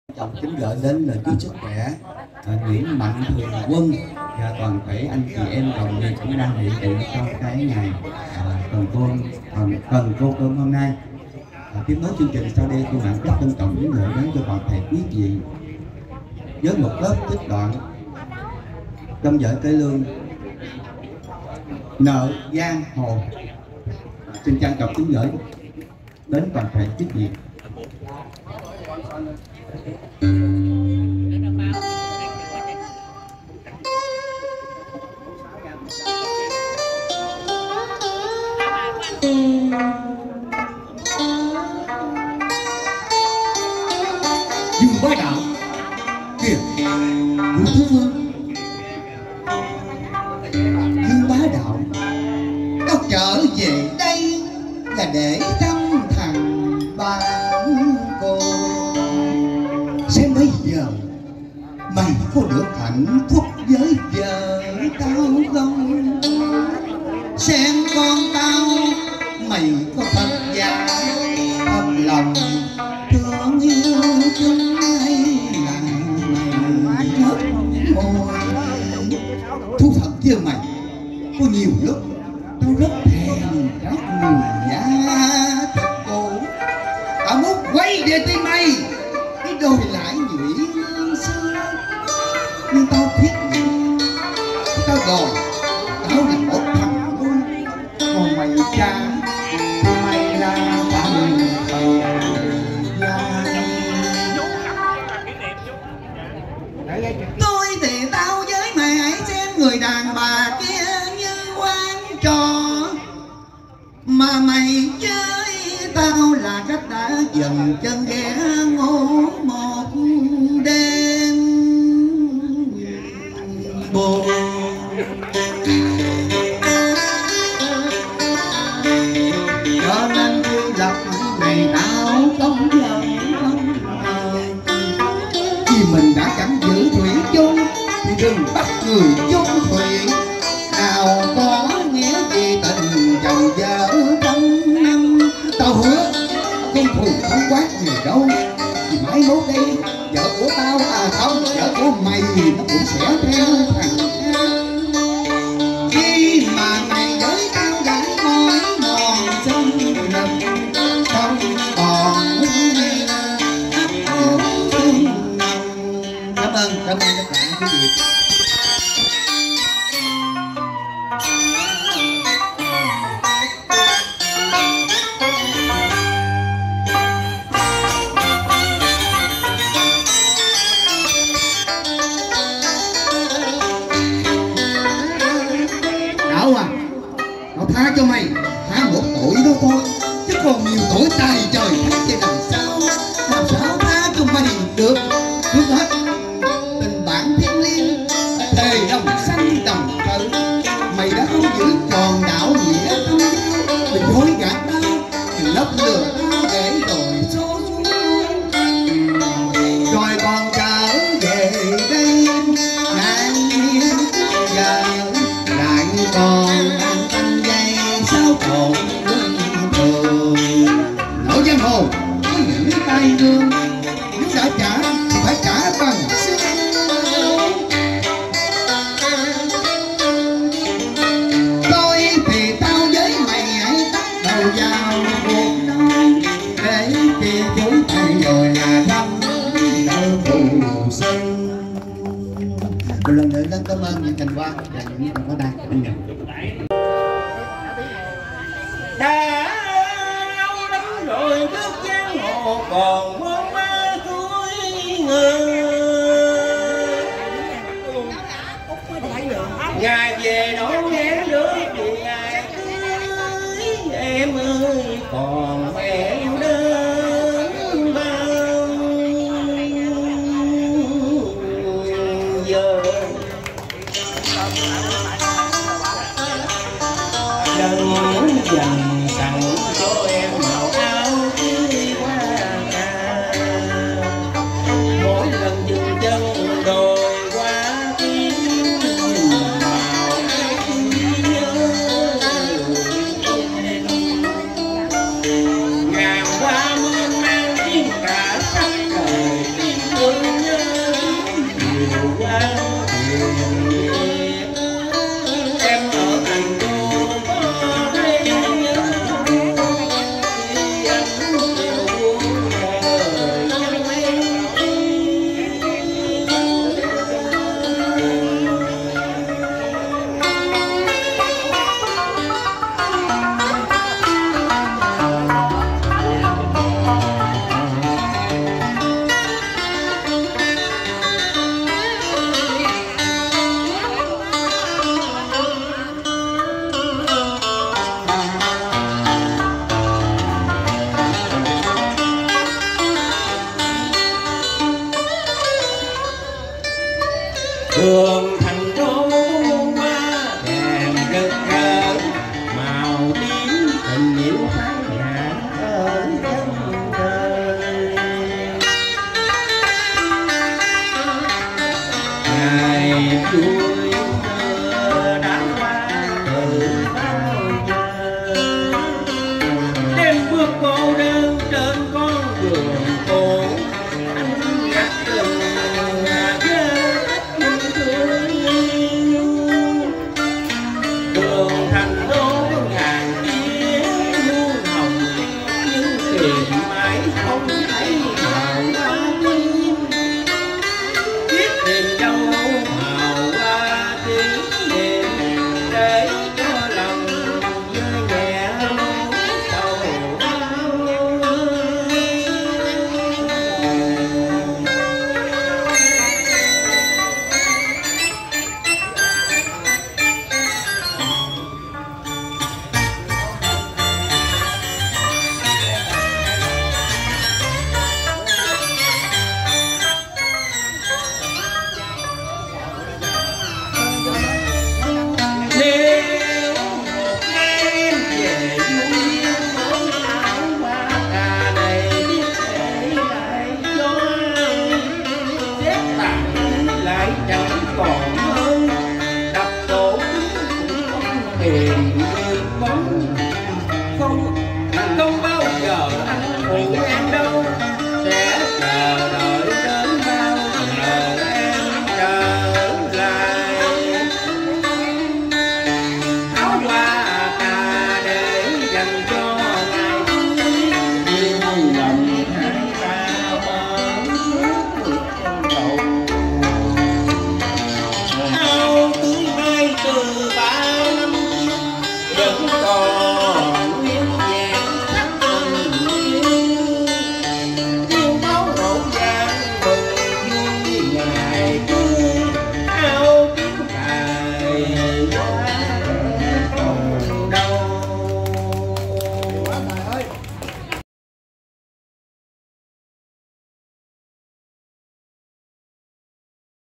NS Minh Minh Cảnh và NS Thành Tài đến với chương trình Trao cần câu cơm cho NSUT Thanh Nguyệt
Sáng ngày 01/3, tại phòng trà Lily, khuôn viên nhà hát Hòa Bình đã diễn ra chương trình Trao cần câu cơm lần thứ 41 cho NSUT Thanh Nguyệt và nghệ sĩ Quốc Nhĩ. Tổng số tiền được trao cho cô chú gần 35 triệu và 400 USD.
Chương trình do nhà báo Trần Đại Phú (Khổ Gia Trường) khởi xướng, nhằm giúp đỡ, hổ trợ những nghệ sĩ, công nhân hậu đài... Có hoàn cảnh khó khăn... NSMinh Minh Cảnh và NS Thành Tài đến tham dự và hát giao lưu với chương trình...